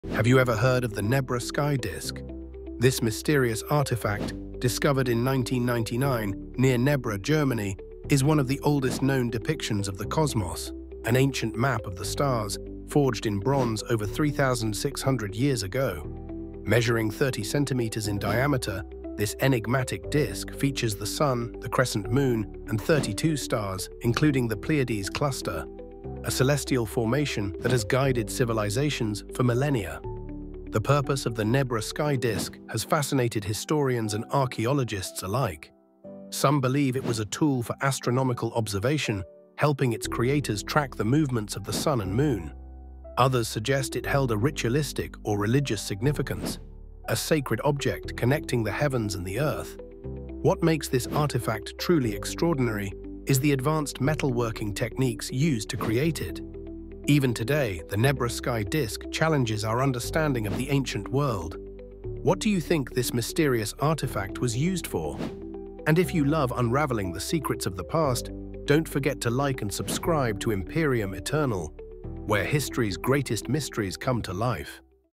The Nebra Sky Disk: A 3,600-Year-Old Map of the Stars
You can create faceless videos like this too using Syllaby!

Discovered in 1999 near Nebra, Germany, the Nebra Sky Disk is one of the oldest known representations of the cosmos, dating back to 1600 BCE. This mysterious bronze artifact features the sun, moon, and stars, including the Pleiades cluster, and showcases advanced metalworking and astronomical knowledge far ahead of its time. Was it an ancient star map, a ritual object, or something else entirely?